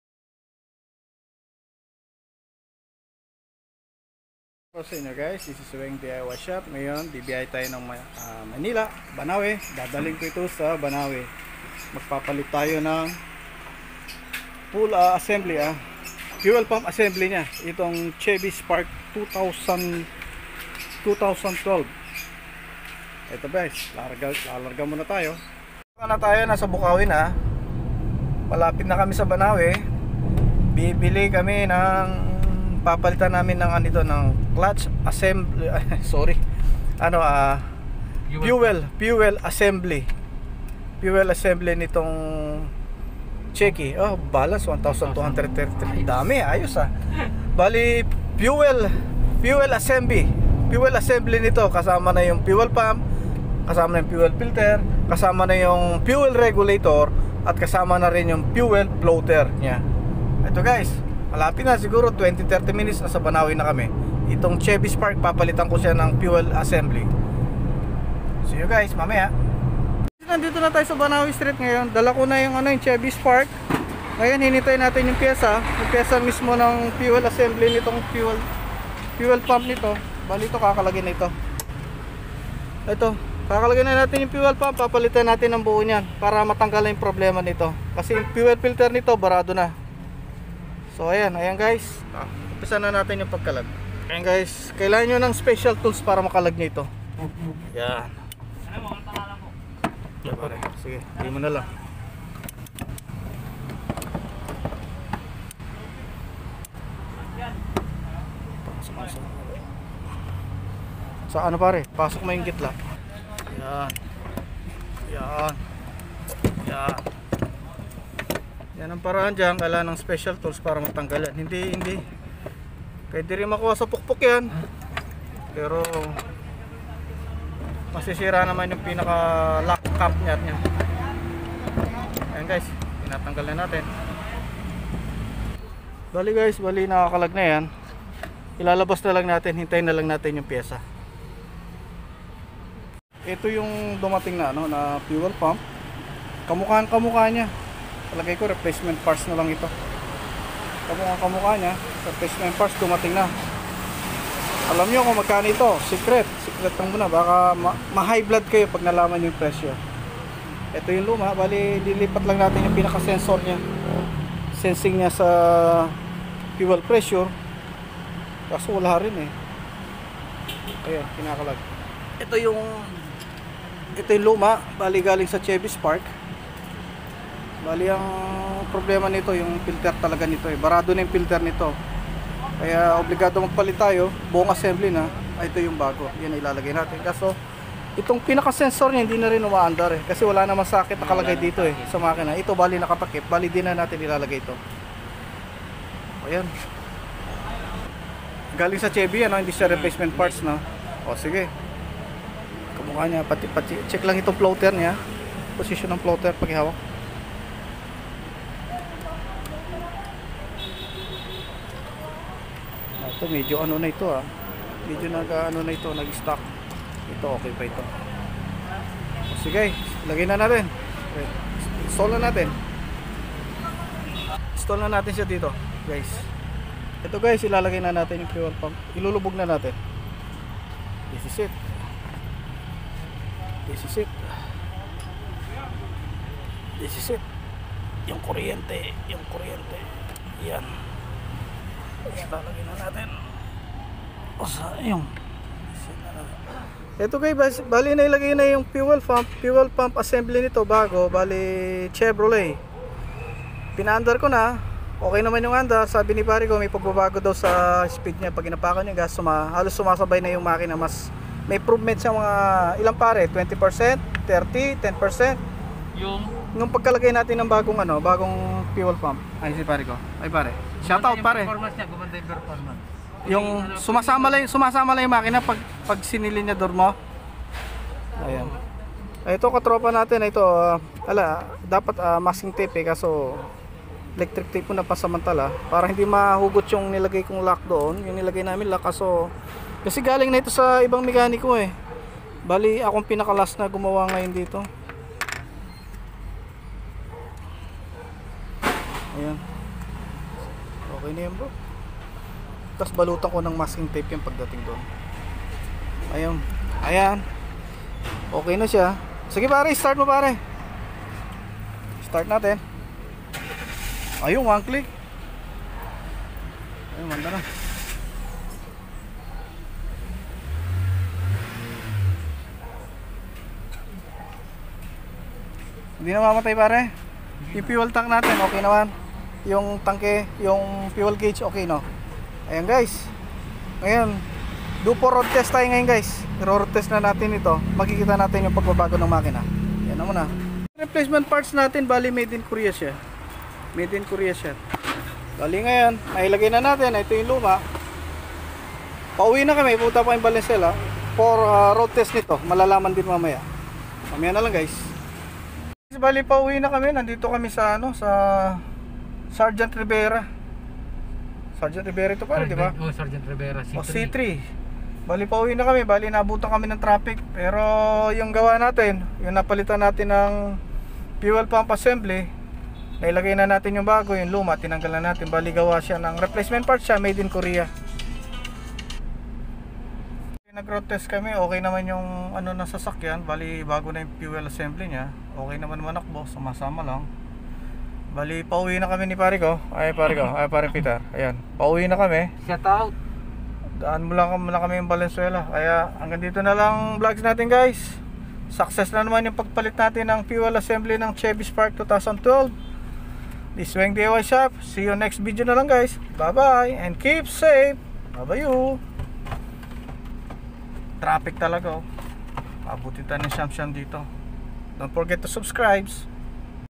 Fuel Pump Assembly Replaced on Chevy Spark 2012
problem,,,,,,, clogged filter / barado ,,,,low power,,,,,,,medyo hirap makina,,,,,,,,, walang filter na mabili kya replaced fuel pump whole assembly,,,,,,,,,,,,, success guysss,,,, please subscribe my YouTube channel enjoy watching guys tnxx n babayyyyy 😜😂🤮😫💛🙄🙄💛💛🙄